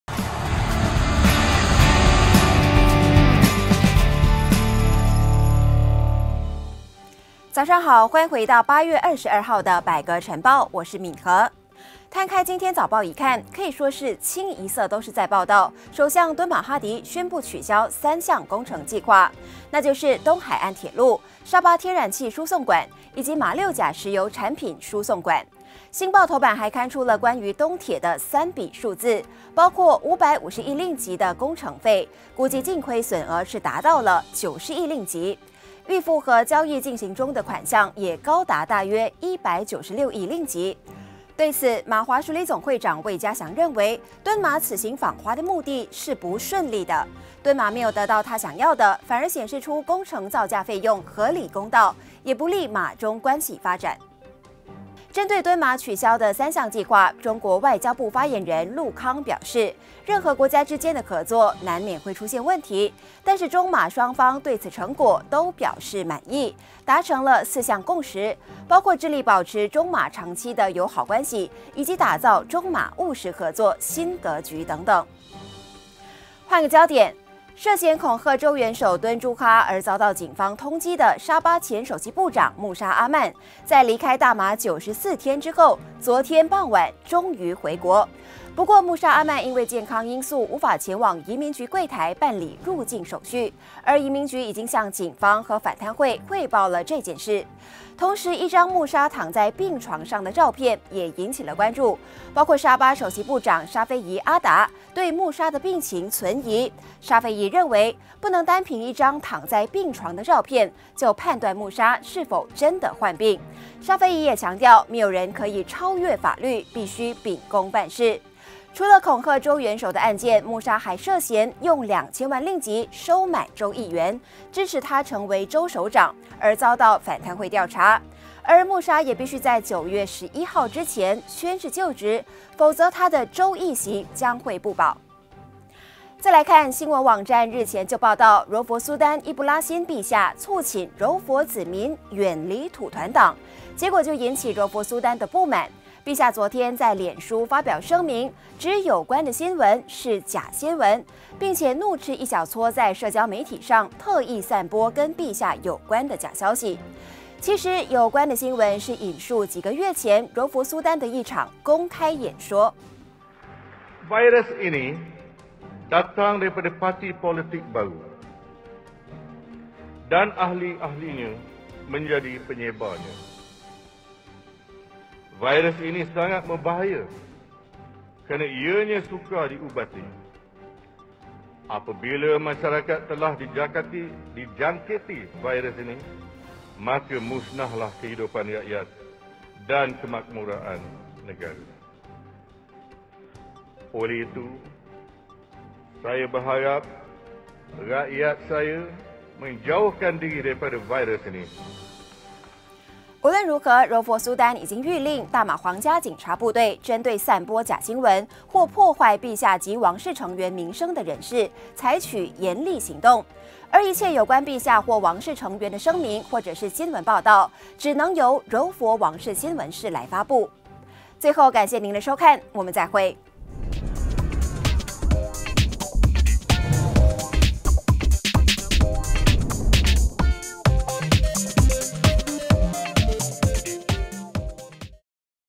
"没指示远离土团党" 柔苏丹震怒要彻查
柔佛苏丹对新闻网站一则假新闻感到震怒，并要求警方彻查.